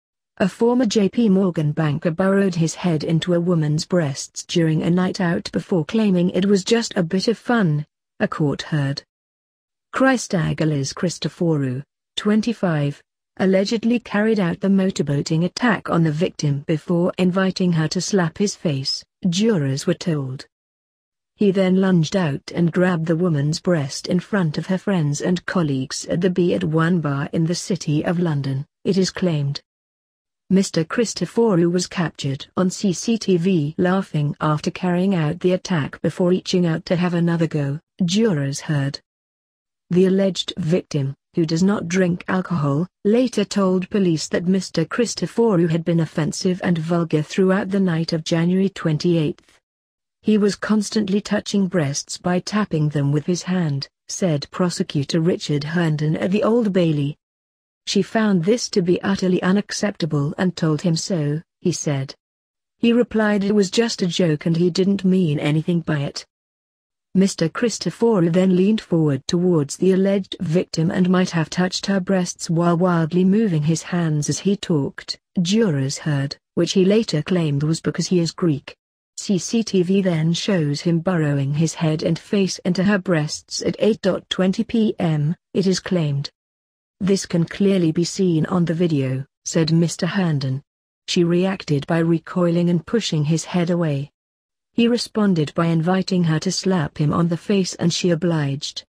Banker caught on CCTV 'motorboating' Womans breasts at London Bar
A former JP Morgan banker burrowed his head into a woman's breasts during a night out before claiming it was "just a bit of fun", a court heard.

Christaggelis Christoforou caught on camera  'motorboating' in Public Bar